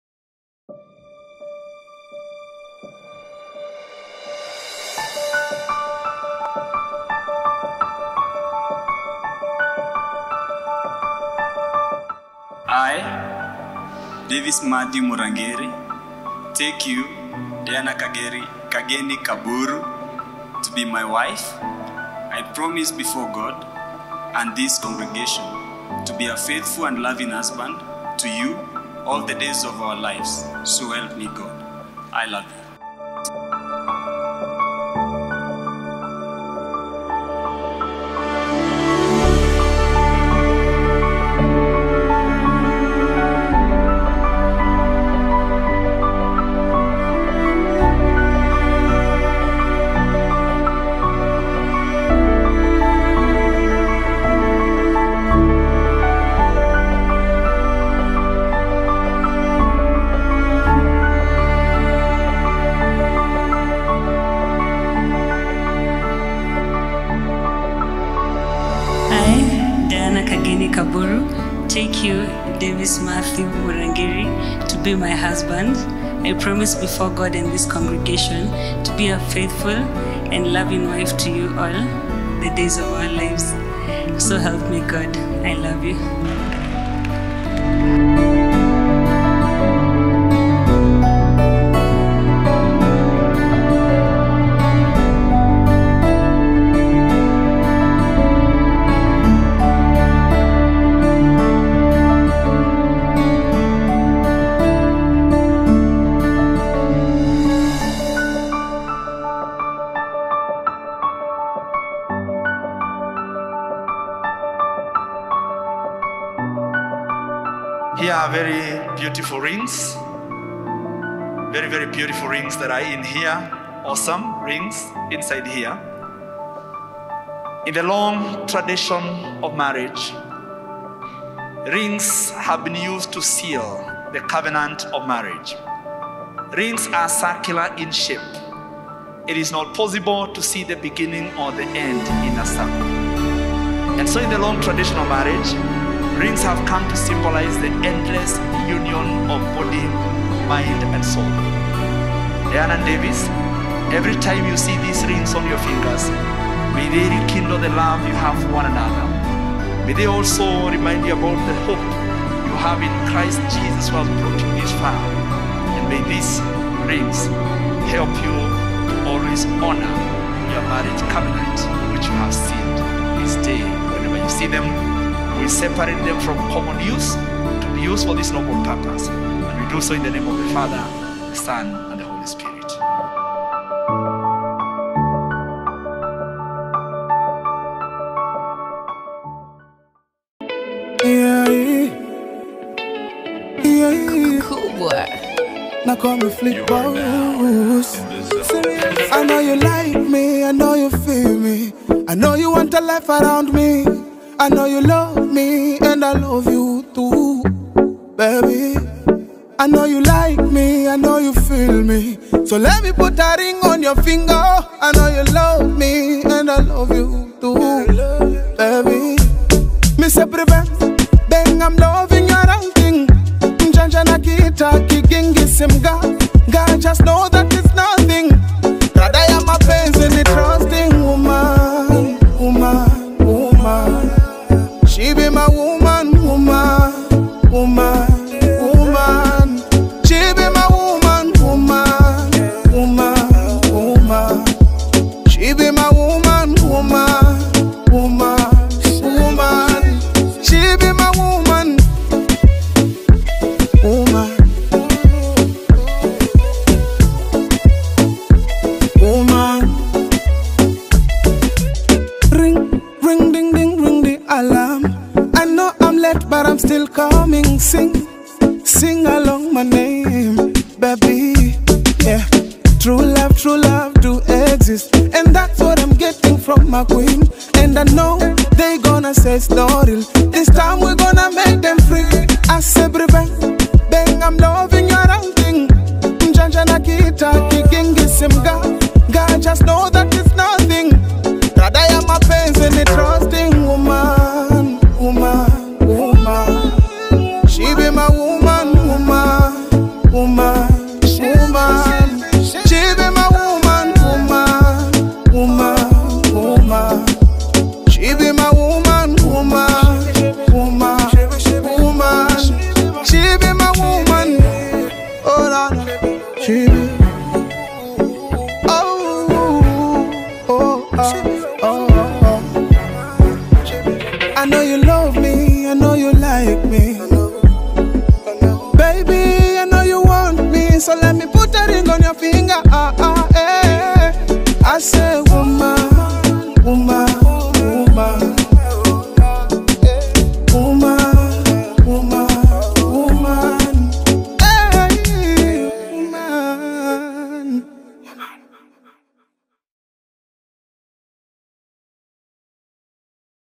Diana & Davis Wedding Trailer
The love story of Diana & Davis who met while in law school. Their day was very special as they celebrated exchange of vows by both family and friends.
Wish them a fulfilling marriage. Cheers